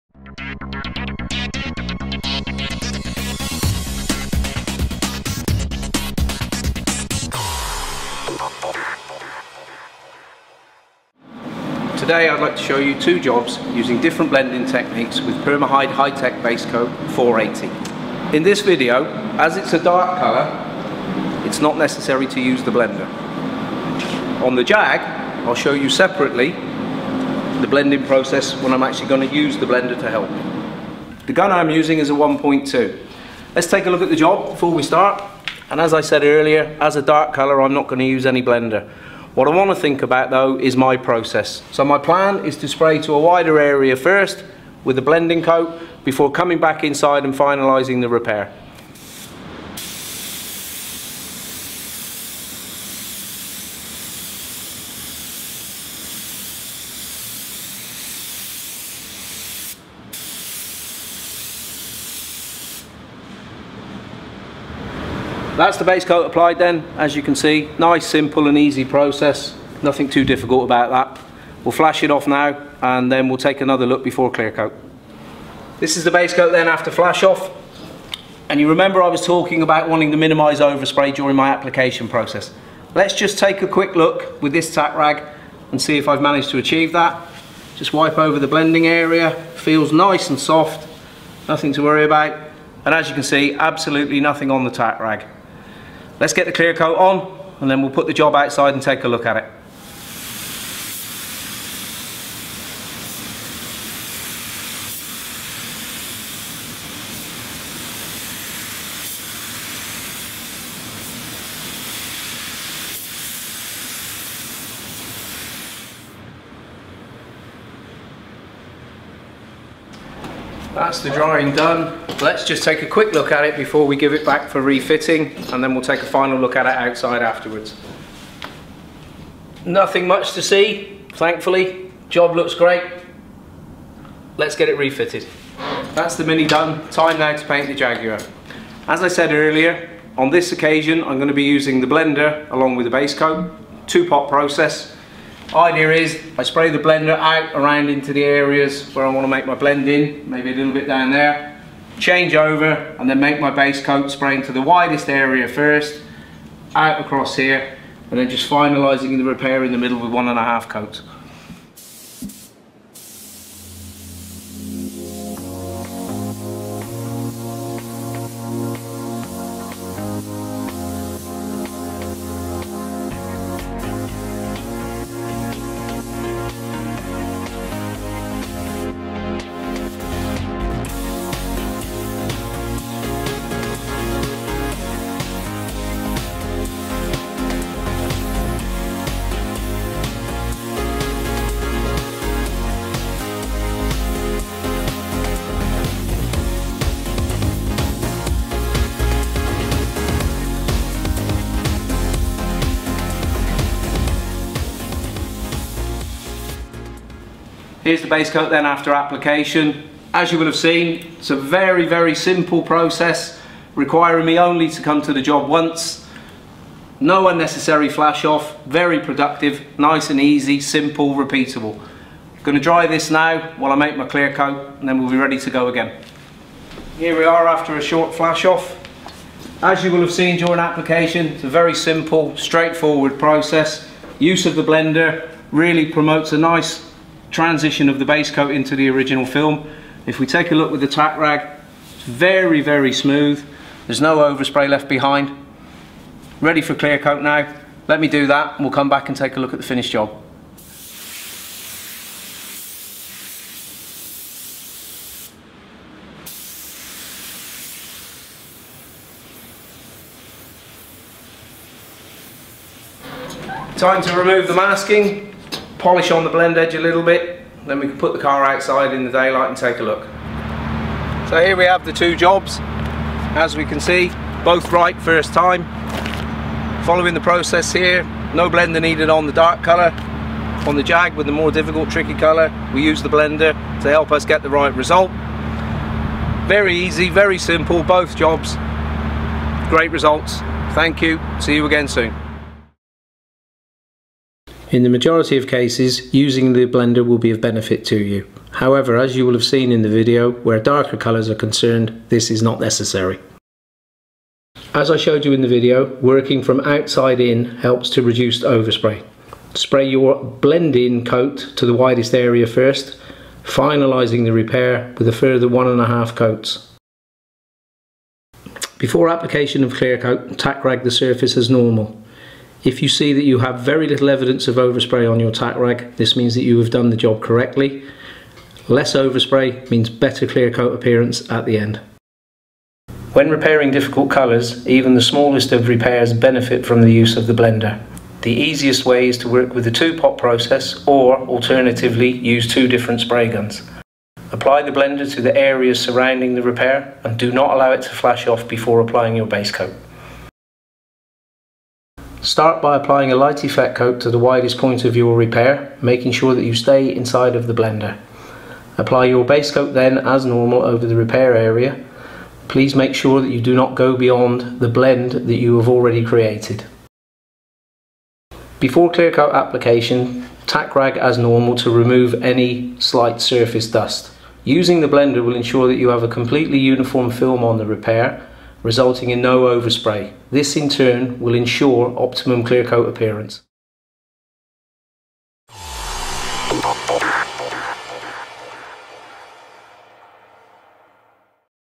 Permahyd Hi-TEC 480: Disfarce com ou sem Aditivo de Disfarces 1050/1051
Sabemos que ao trabalhar com Permahyd Hi-TEC - o Aditivo de Disfarces é usado apenas algumas vezes. Neste breve vídeo, mostraremos quando deve usar o aditivo de disfarces e quando pode fazer a sua pintura sem ele. Veja também os nossos outros vídeos para fazer disfarces em áreas maiores e como repintar cores Tricamada.